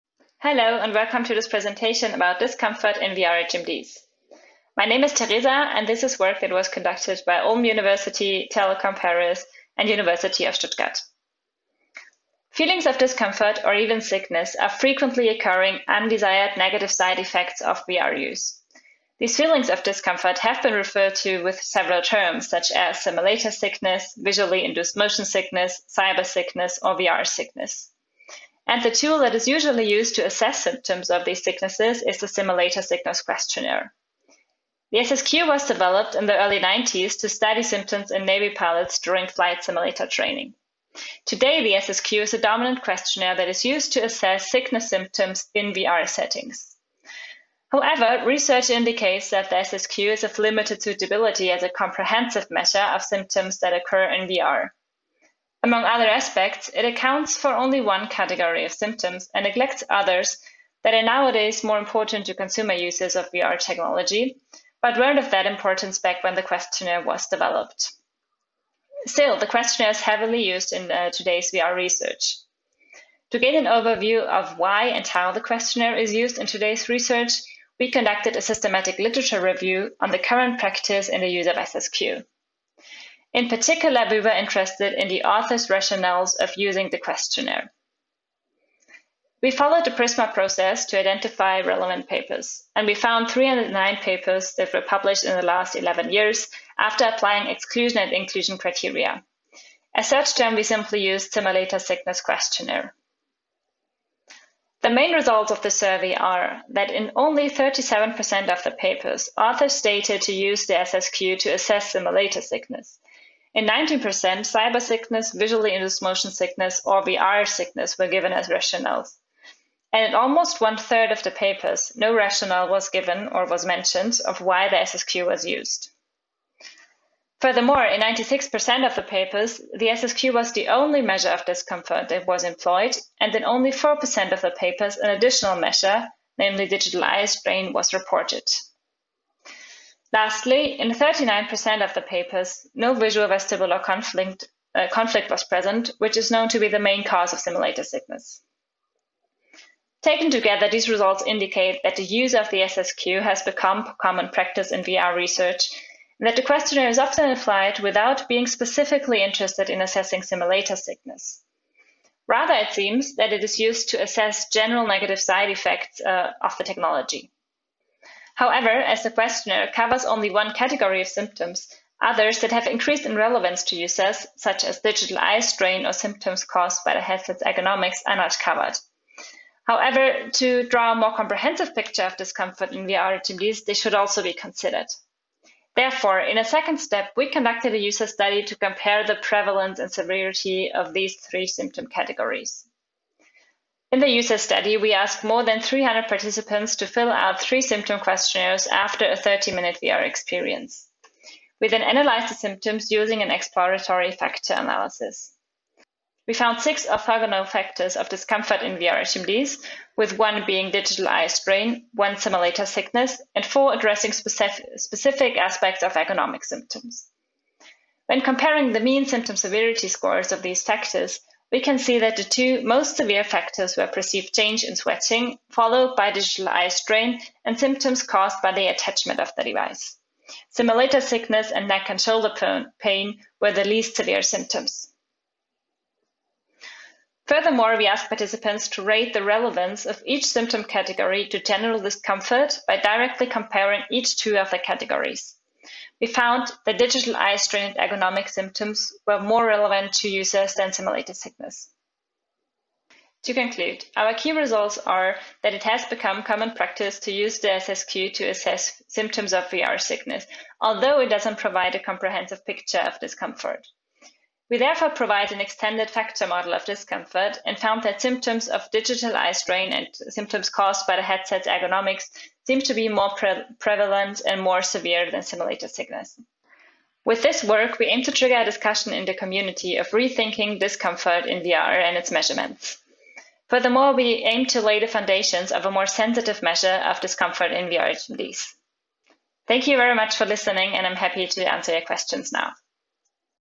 CHI 21 A Critical Assessment of the Use of SSQ as a Measure of General Discomfort in VR HMDs
Paper presentation video at CHI '21.
Paper: A Critical Assessment of the Use of SSQ as a Measure of General Discomfort in VR Head-Mounted Displays
Authors: Teresa Hirzle, Maurice Cordts, Enrico Rukzio, Jan Gugenheimer, and Andreas Bulling